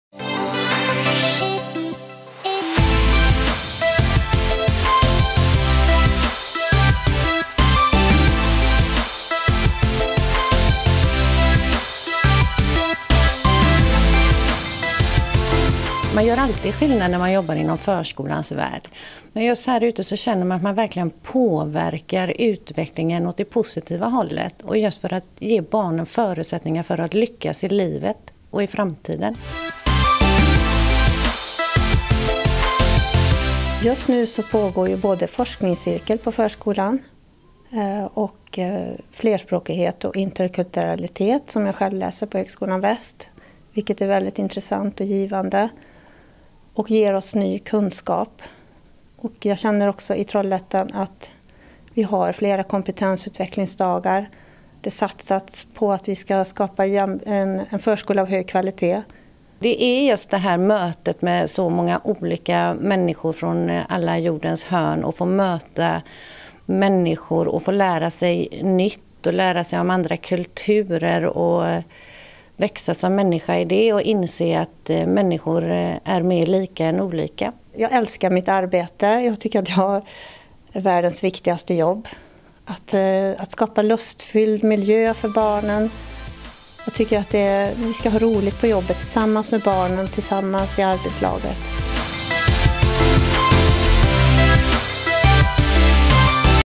Vi söker förskollärare
Trollhättans Stad söker förskollärare till våra verksamheter. Se filmen där två pedagoger beskriver sitt arbete.